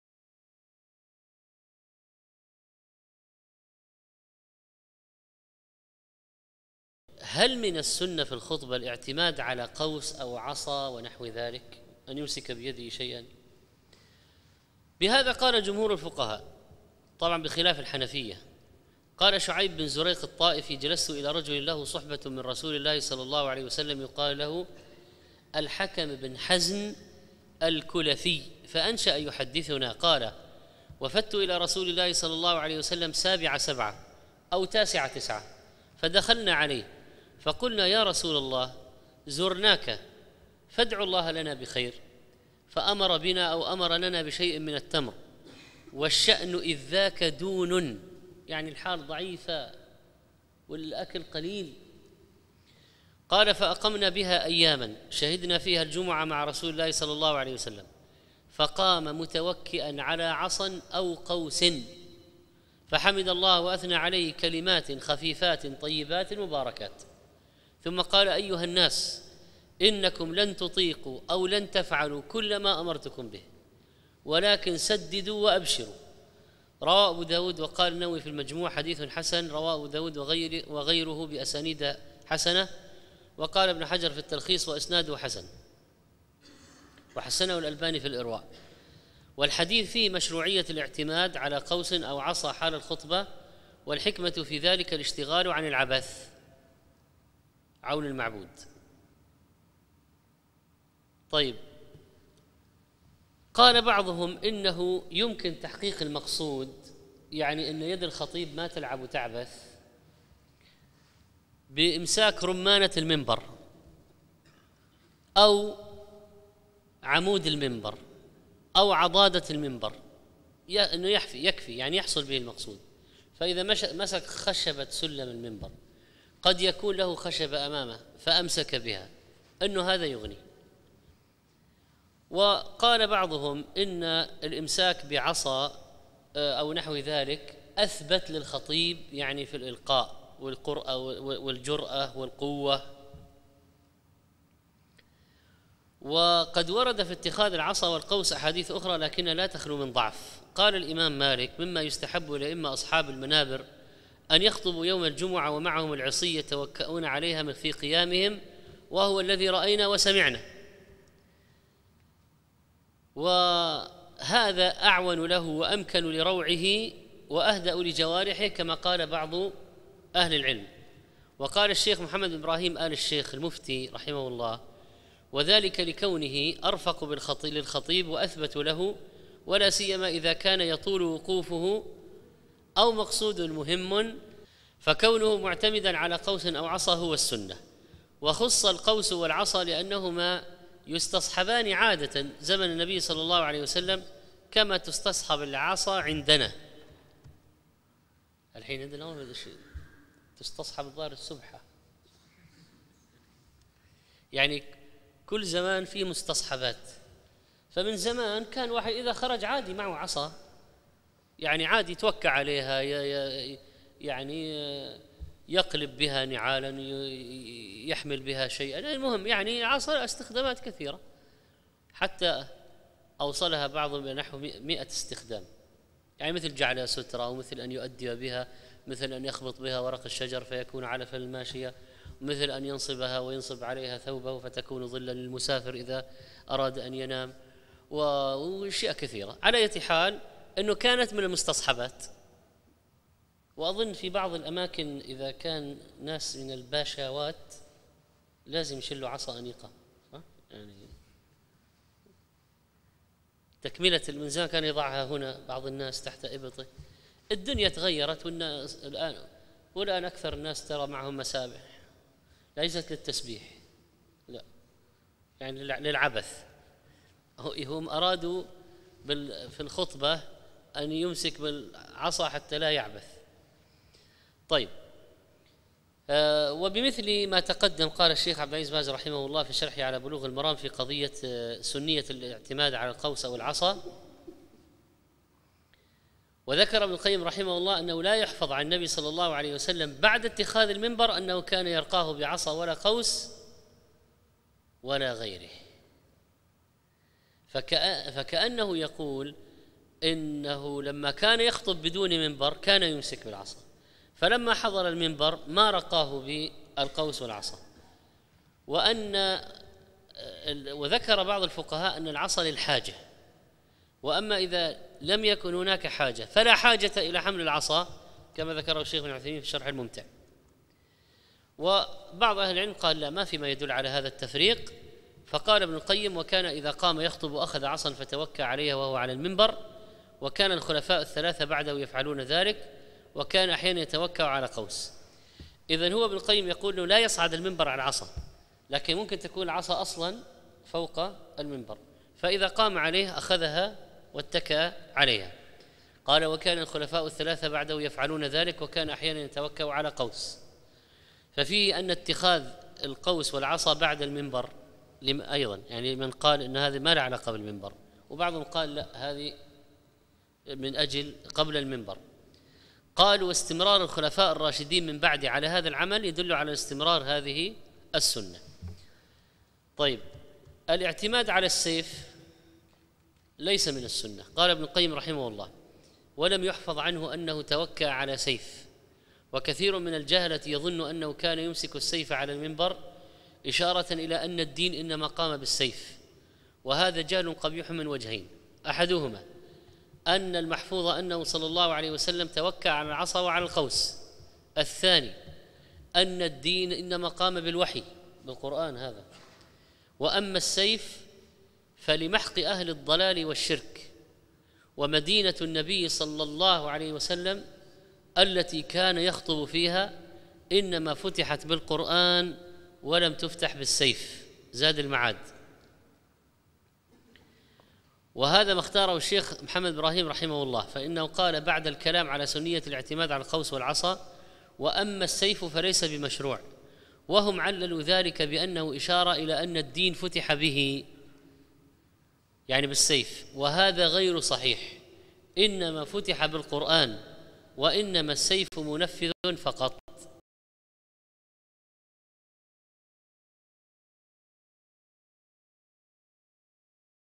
هل من السنة الاعتماد على قوس أو عصا ونحو ذلك في الخطبة؟
مسألة من درس أحكام صلاة الجمعة - الدرس السابع من سلسلة منتقى المسائل الفقهية من منهج السالكين من شرح منهج السالكين للشيخ عبد الرحمن بن سعدي رحمه الله تعالى شرح الشيخ محمد صالح المنجد حفظه الله تعالى